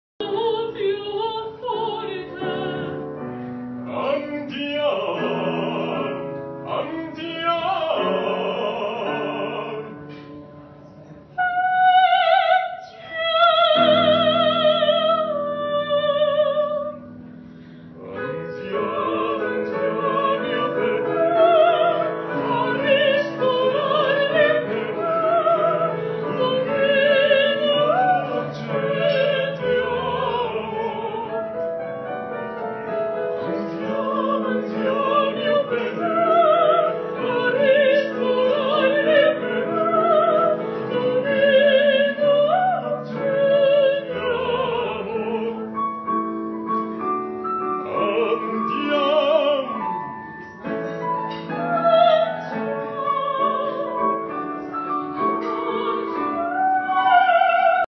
Girlahead at Cusco Belmond Monasterio
Stunning opera dinner Tuesday, Thursday and Saturday